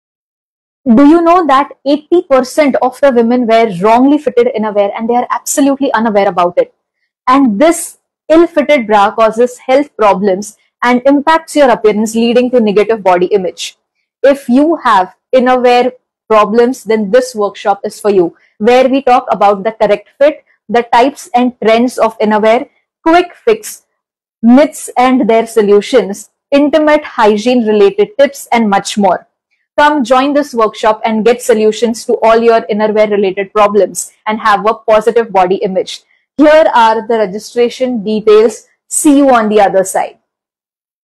Importance of wearing the right lingerie | How bra can affect our body?
Wearing an ill-fitted bra can lead to several problems and discomfort. Here are some of the issues that can arise due to an improperly fitting bra:

1. Discomfort: An ill-fitted bra can cause physical discomfort, such as pinching, digging, or poking. The bra straps may be too tight, leaving marks on the shoulders, or they may constantly slip off. The underwire may press against the breast tissue, causing pain and irritation.

2. Poor Support: A bra that does not fit properly fails to provide adequate support to the breasts. This lack of support can lead to sagging, breast pain, and discomfort during physical activities.

3. Back and Neck Pain: When a bra doesn't fit correctly, it can put excessive pressure on the back and neck. This can result in chronic pain, muscle strain, and even headaches.

4. Breathing Problems: If the band of the bra is too tight, it can restrict proper breathing by compressing the rib cage and diaphragm. This can lead to shallow breathing and discomfort.

5. Posture Issues: An ill-fitting bra can affect your posture by pulling the shoulders forward or causing the back to arch. This can result in poor posture and contribute to back pain and muscle imbalances and more.
Enrol for our Intima workshop to understand the art and craft of wearing your lingerie perfectly.